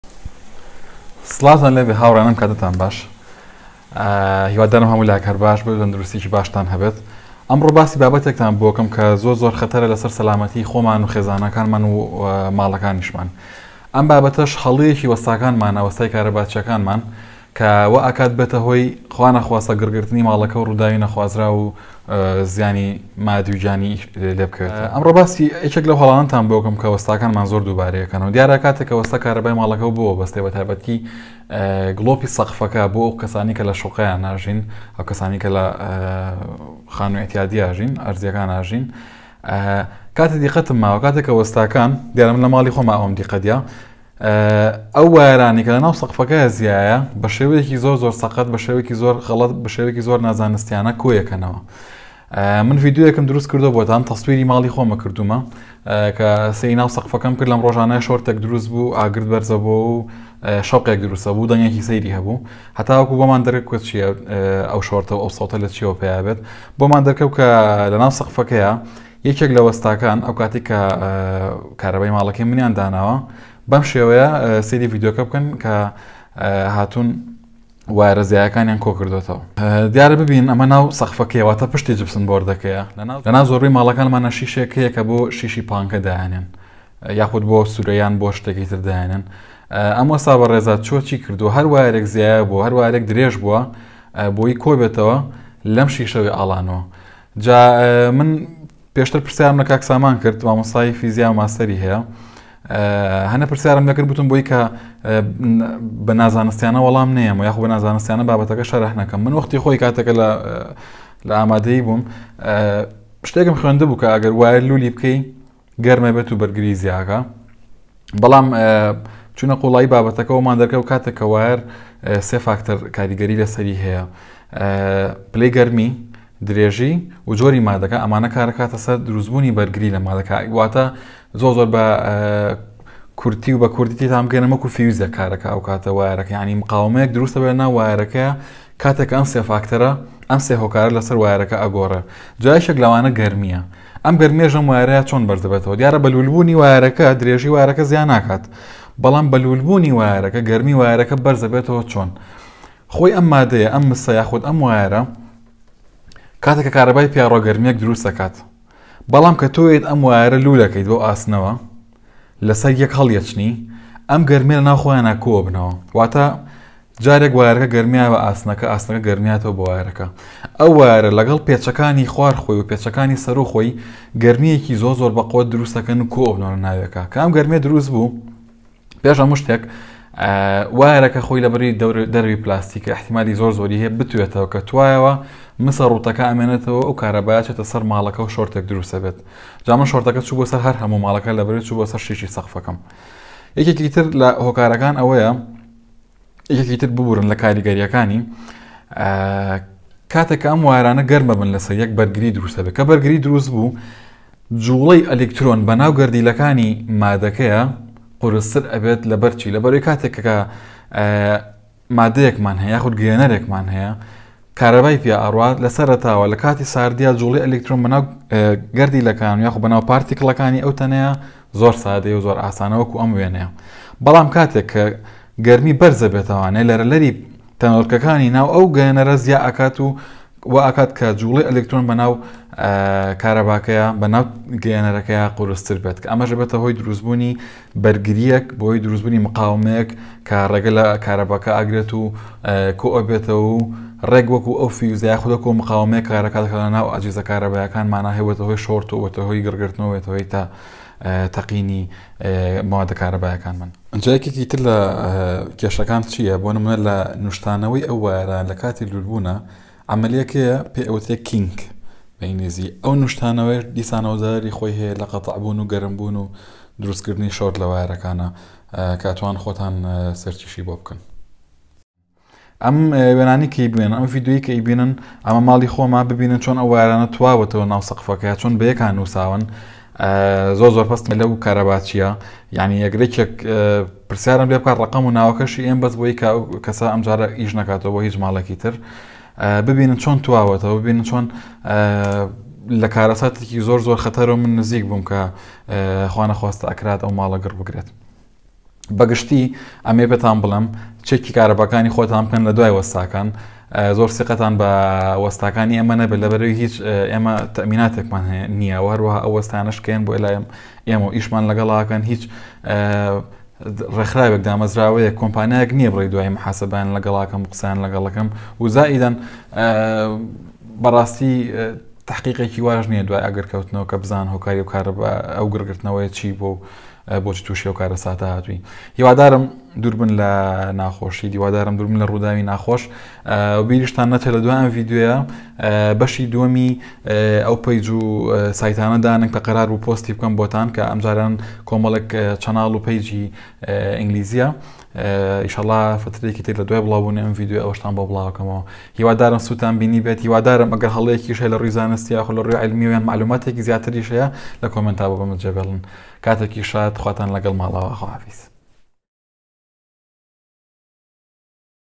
مەترسیدارە زۆر ئاگادار بن CAUTION kinking in Cables and E wires
تکایە ئاگاداری ئەم بابەت بن زۆر مەترسیدارە هەروەها هاوڕێکانتان ئاگادار بکەنەوە